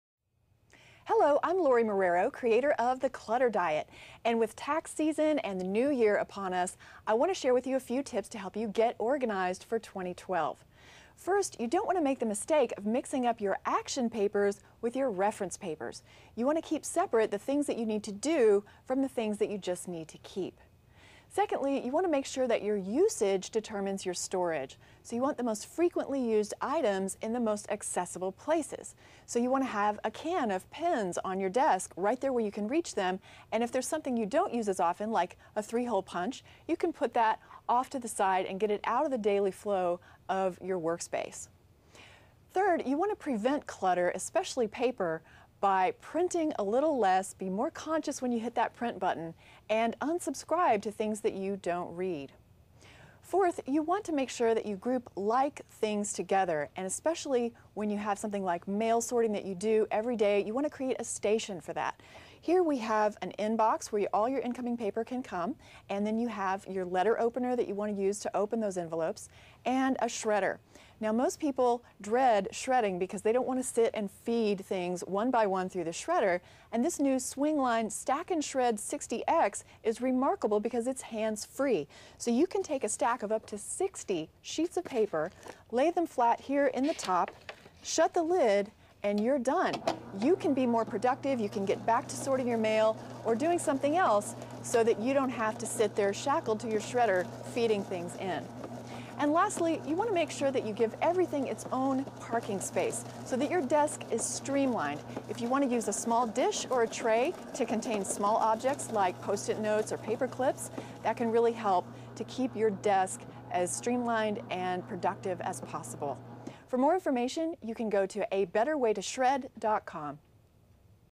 Get Organized with Lorie Marrero! Feat the Swingline™ Stack-and-Shred™
Swingline Expands Stack-and-Shred Family of Products to Cut Your Workload; New Stack-and-Shred 60X Fits Any Size Office and Budget.

Anyway You Cut It, Swingline Stack-and-Shred Office Technology Offers Best Options for Personal or Business Shredding.